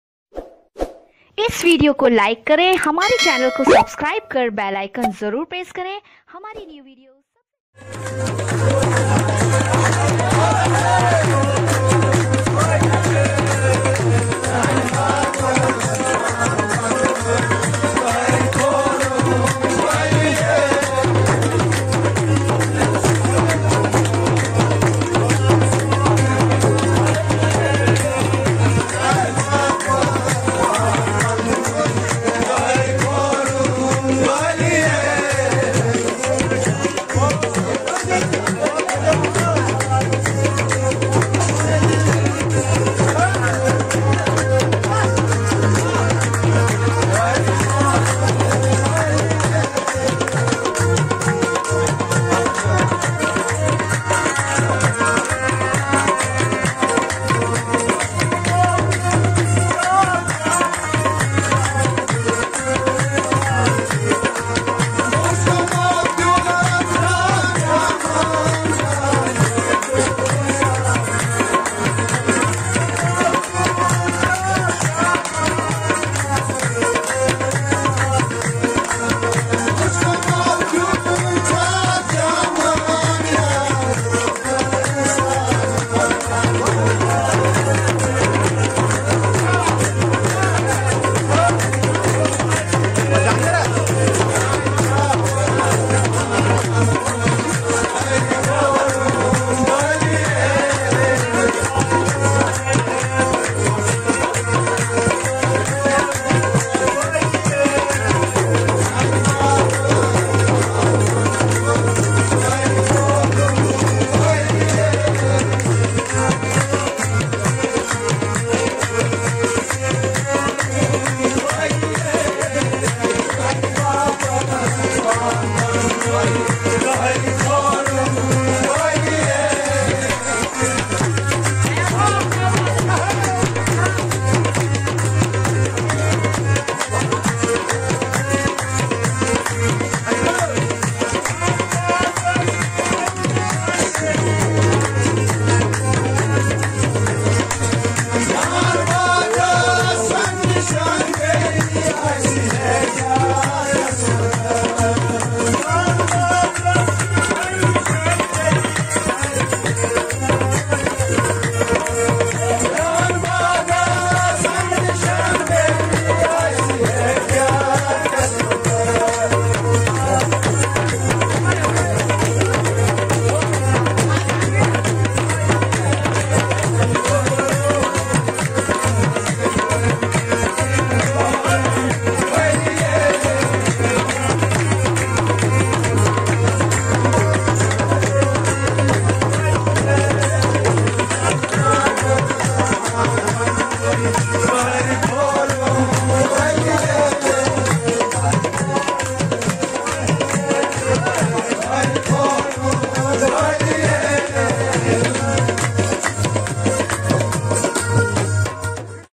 Kashmiri Songs||By Arshad Hussain(Nikka)Kishtwari
Singer||Arshad Hussain (Nikka)

Tabla Master:-Ab.Rashid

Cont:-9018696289